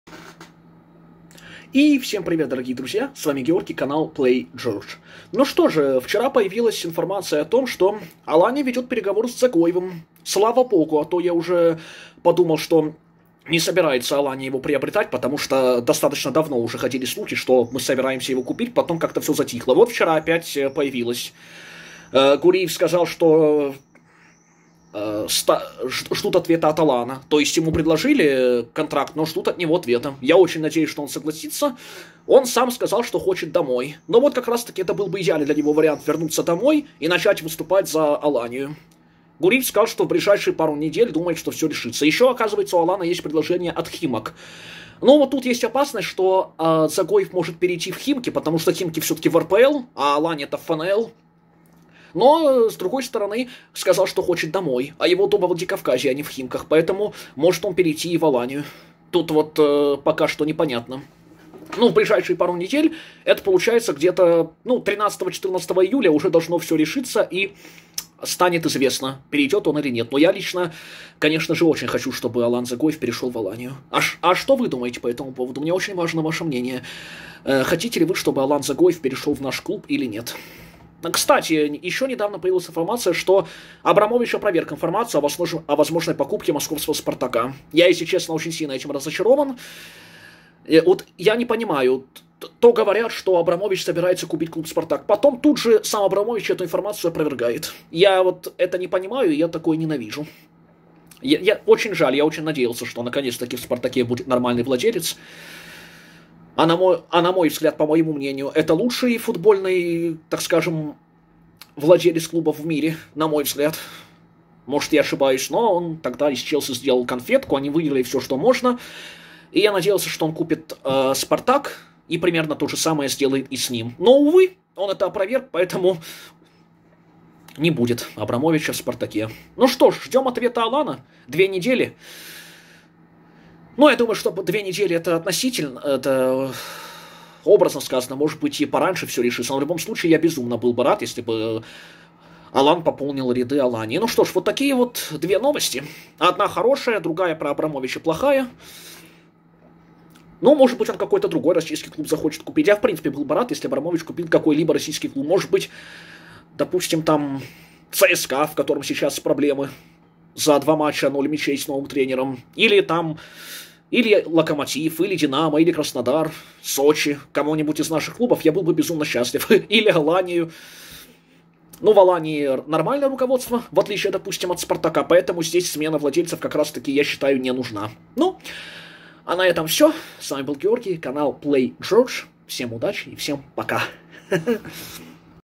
Алания ведёт переговоры с Дзагоевым
Вице-президент Алании Данил Гуриев рассказал,что клуб ведёт переговоры о переходе экс-полузащитника ЦСКА Алана Дзагоева.Данил Гуриев сказал,что Алания ждёт ответа от Дзагоева.Первая реакция Алана:хочу домой.В ближайшие пару дней всё должно решиться сообщил Гуриев.Делюсь своим мнением по этому поводу.